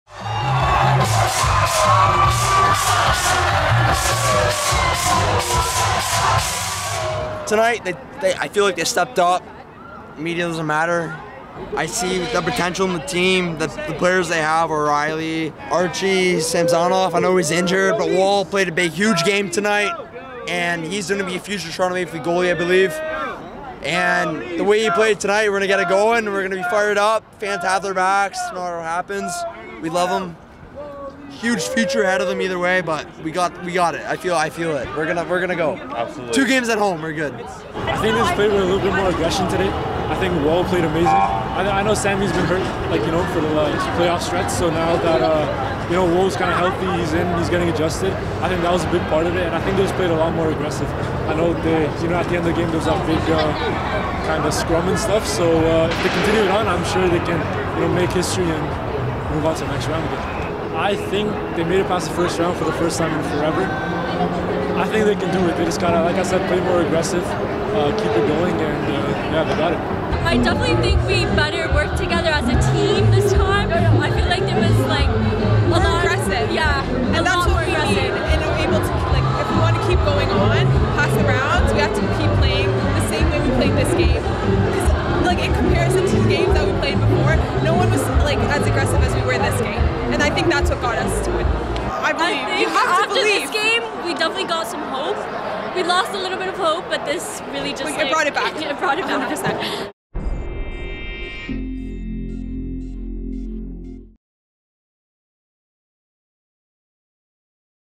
Fans have high hopes as Maple Leafs live to play another day
Maple Leafs fans who gathered in downtown Toronto to watch the hockey team battle for NHL playoff survival were rewarded for their faith and loyalty on Wednesday night. Hundreds of fans in Maple Leaf Square outside the Leafs' home rink Scotiabank Arena were jubilant after the team held out a 2-1 win against the Florida Panthers to cut their second-round series deficit to 3-1 and stave off elimination.